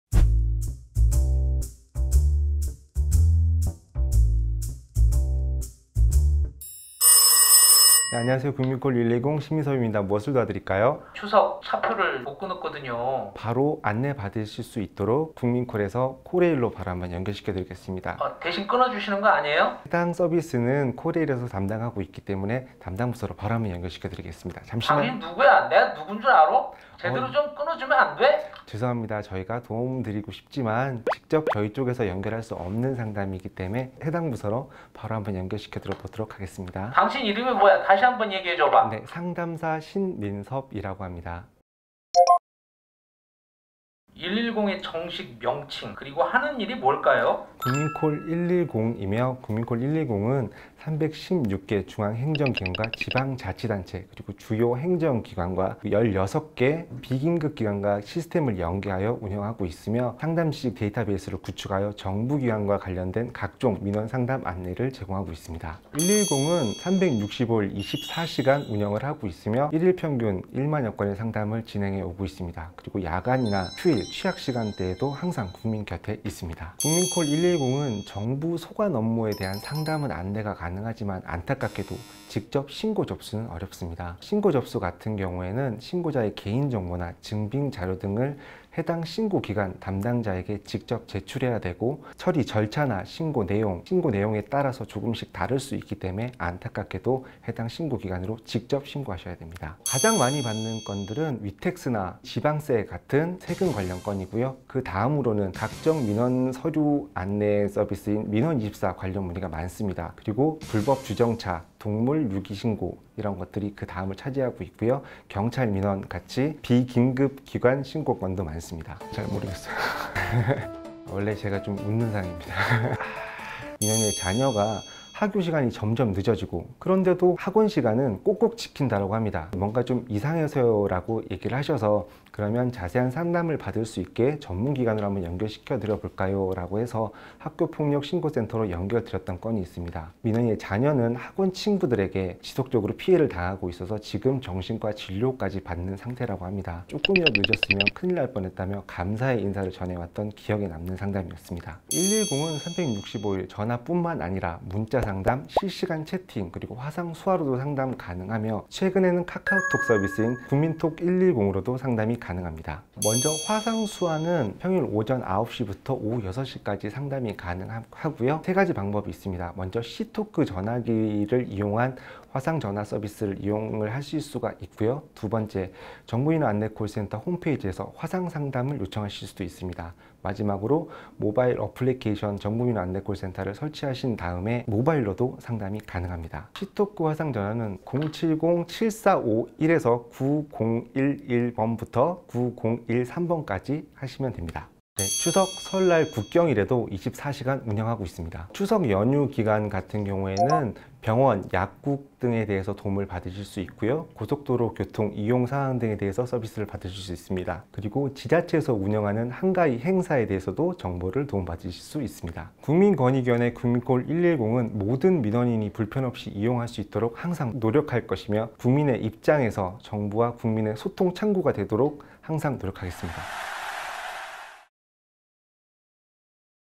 추석 연휴에도 국민콜 110에 무엇이든 물어보세요~!
국민콜 110에 대한 모~든 것을 알려드립니다 ^_^
명절 연휴에도 국민콜은 쉬지 않고
병원, 약국, 고속도로 정보 등을 알려드리니
무엇이든 물어보세요~!